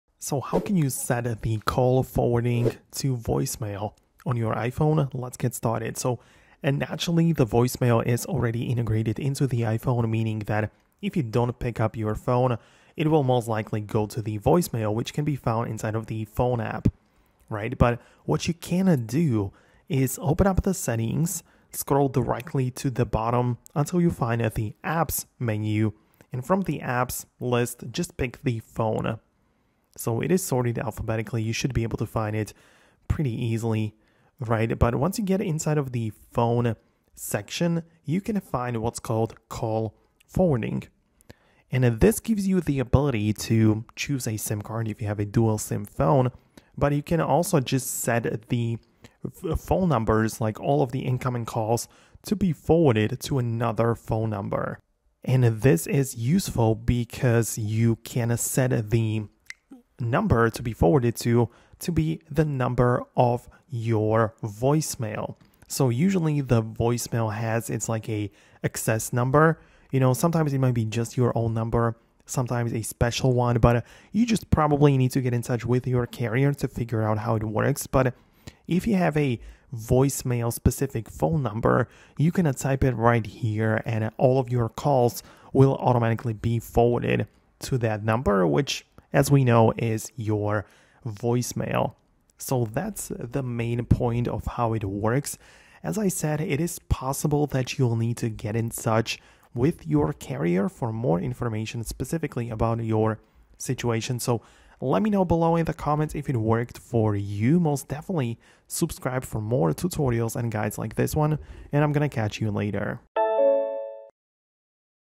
How to Set Call Forwarding to Voicemail in iPhone (tutorial)
Want calls to go straight to voicemail on your iPhone? This video walks you through how to set up call forwarding so unanswered calls are redirected directly to voicemail.